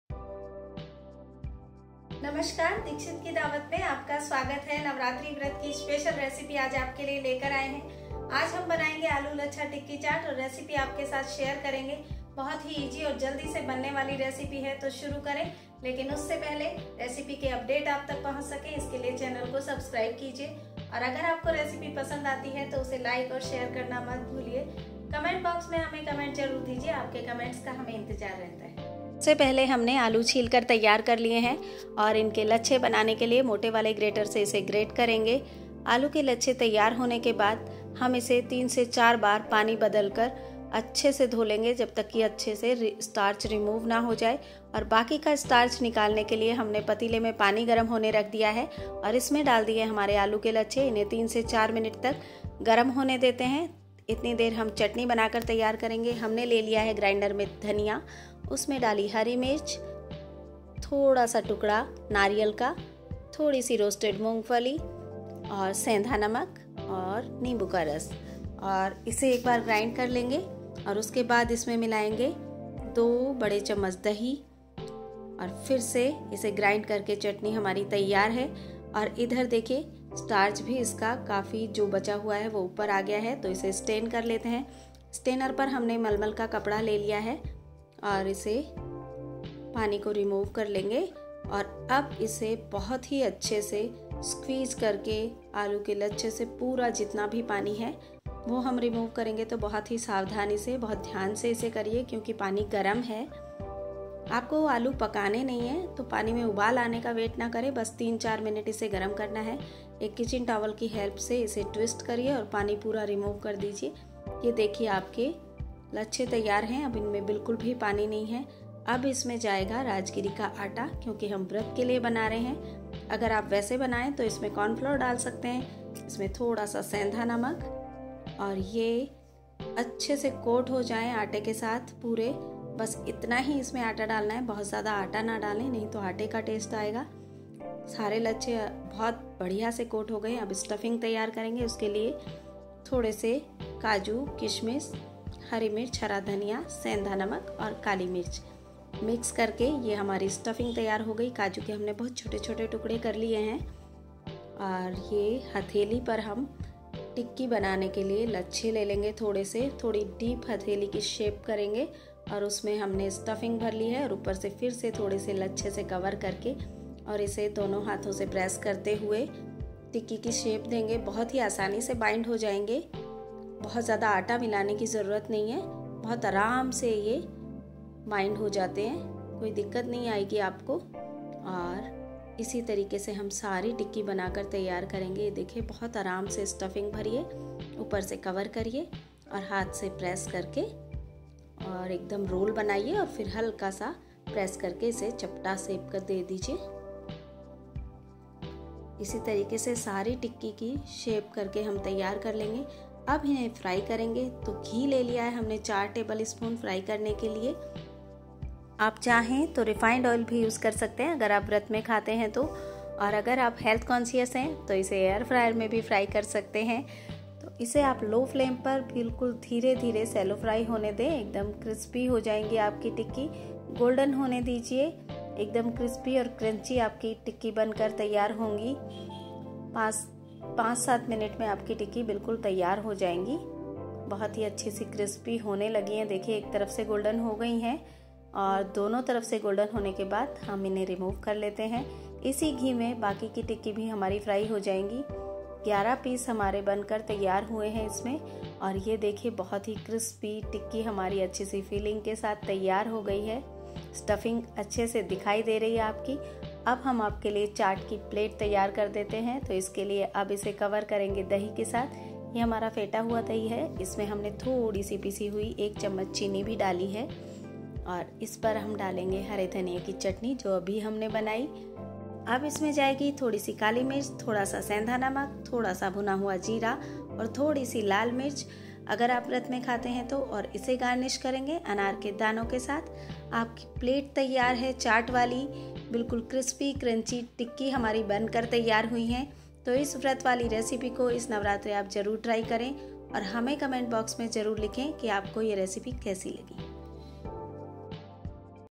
Aaloo Lachcha Tikki Chaat
In this video we are sharing with you the easy and tasty recipe of Aaloo Lachcha Tikki Chaat !!!

Ingredients:
[ Serves- 11 pieces ]
⭐For tikki
✔️6 potato
✔️3tbsp Rajgiri aata
✔️Rock salt to taste
⭐For stuffing
✔️1tbsp Chopped cashew
✔️1tbsp resins
✔️1tbsp Chopped green chilli and coriander
✔️Black pepper
⭐For frying
✔️4tbsp ghee
⭐For chutney
✔️ Coriander leaves
✔️Green chilli
✔️Small coconut piece
✔️1tbsp roasted peanuts
✔️½ lemon
✔️2tbsp curd
⭐ To serve
✔️Green chutney
✔️ Beaten curd +1tsp powdered sugar
✔️Black pepper
✔️Rock salt
✔️Roasted jeera
✔️Red chilli powder
⭐Garnish with
✔️ Pomegranate seeds